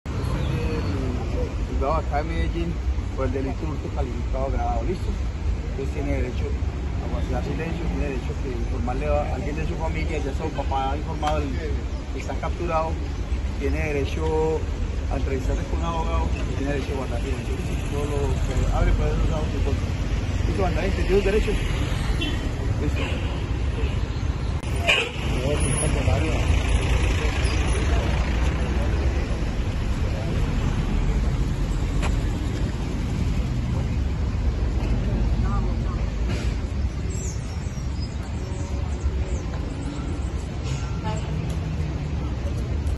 Hurtos en Medellín se redujeron en 14 % en los últimos 40 días
En los primeros 40 días de acciones estratégicas, articuladas entre la Policía Metropolitana, la Fiscalía y el Sistema Integrado de Emergencias y Seguridad de Medellín (SIESM) y en el marco del Plan de Choque 360°, se revelaron los primeros resultados en la ciudad contra el hurto en todas las modalidades, en particular contra el hurto a personas, que se redujo en 14 %. Son en total 543 casos menos comparado con el periodo inmediatamente anterior.